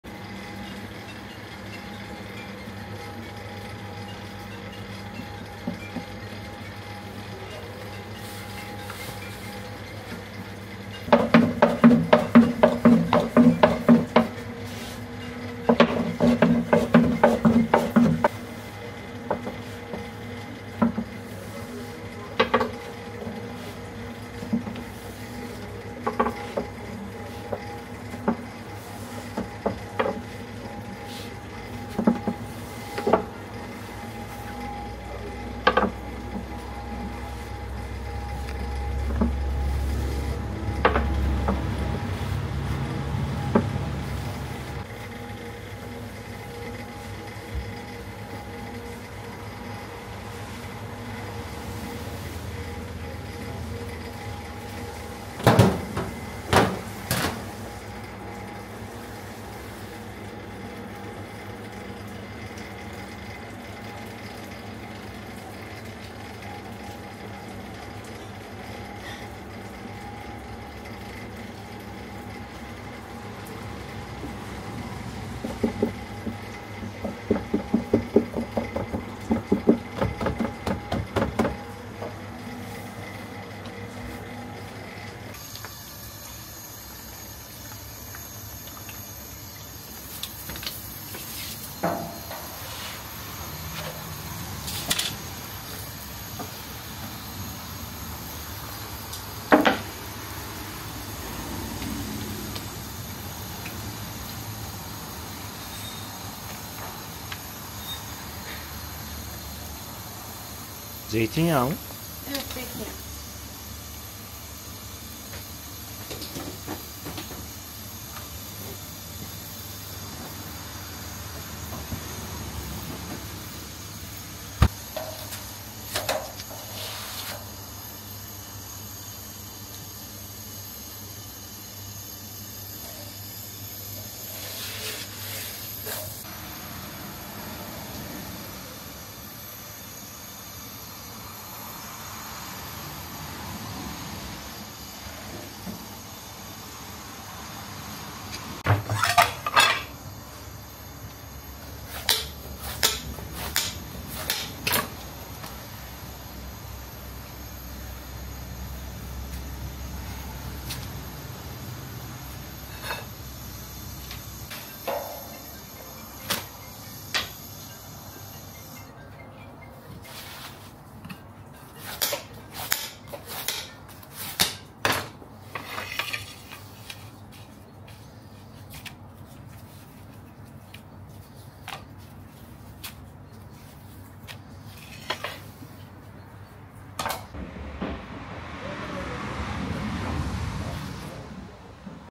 How to Make Pancake in Türkiye? | Traditional Recipe from the Masters!
In this video, learn how to make Turkey's beloved pancake from the masters themselves! Witness the step-by-step process of preparing this delicious treat using traditional methods. Discover the secrets of making pancake and explore various fillings.